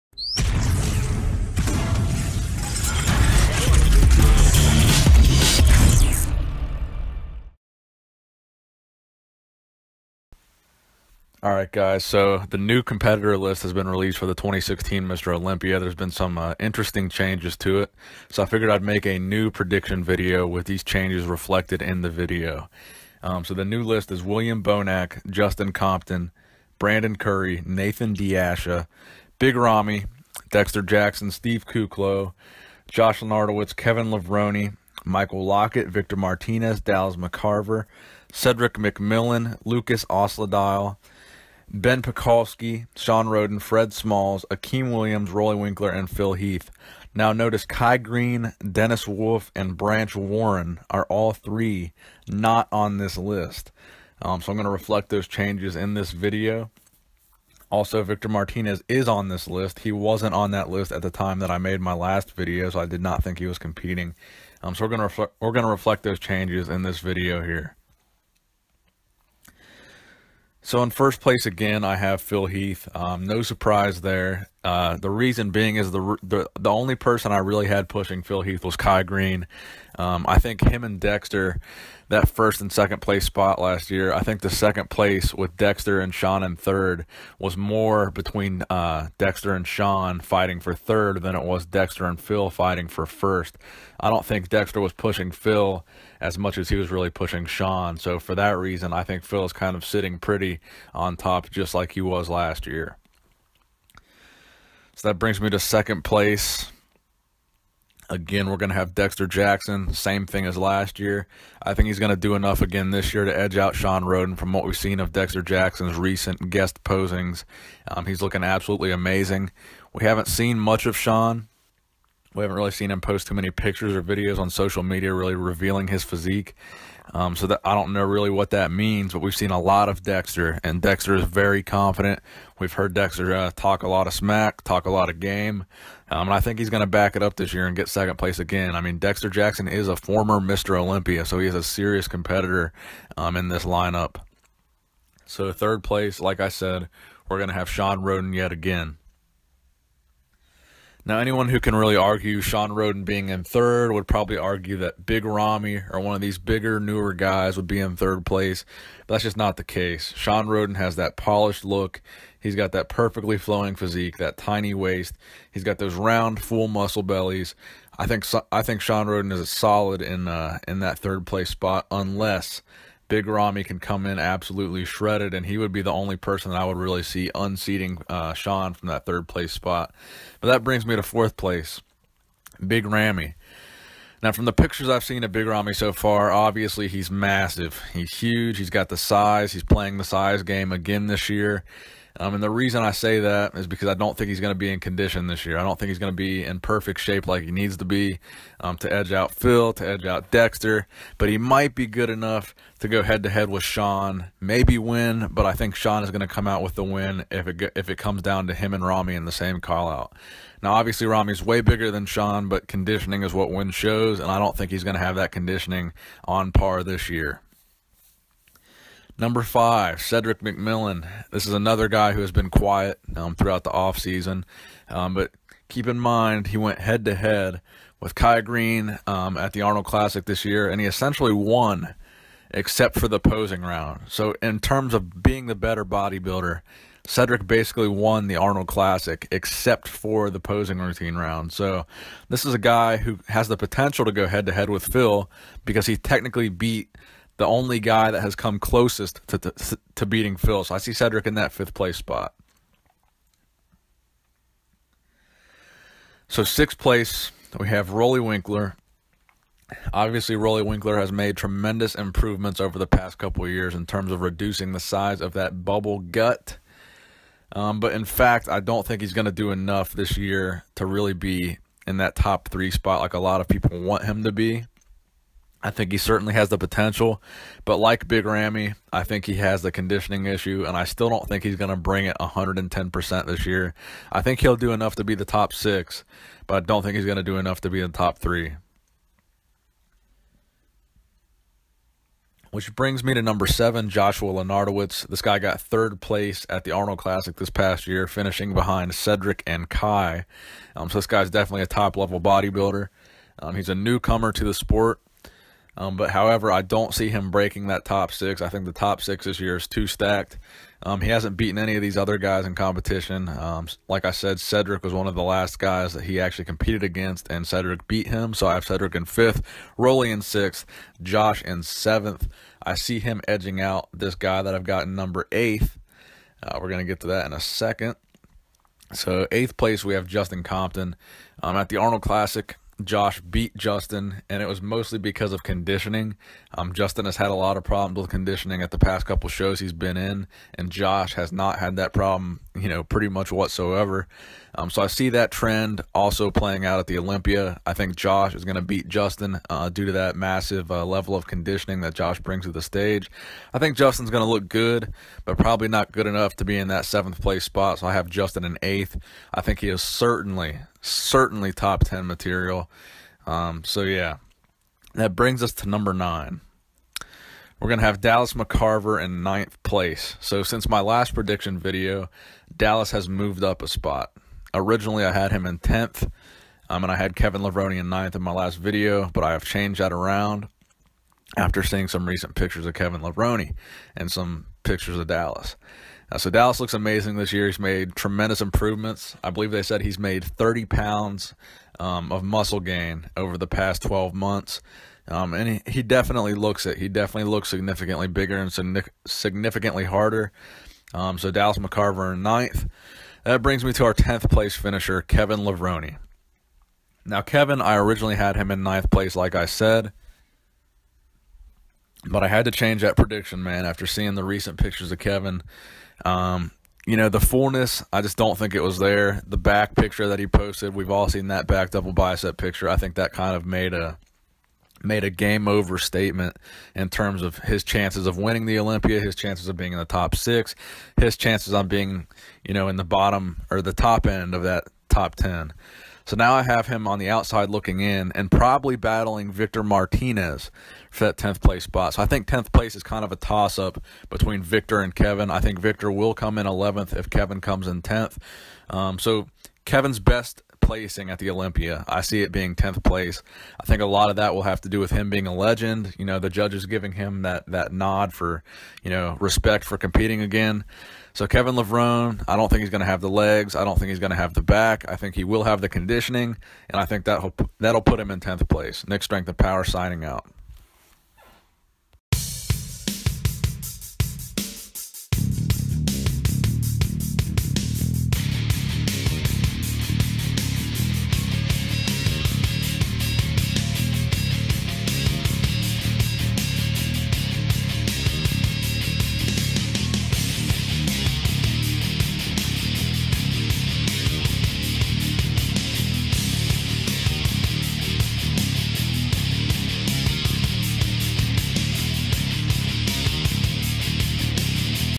2016 Olympia Top 10 Predictions (updated)
My updated 2016 Mr. Olympia Predictions, using the official competitor list 1 week out.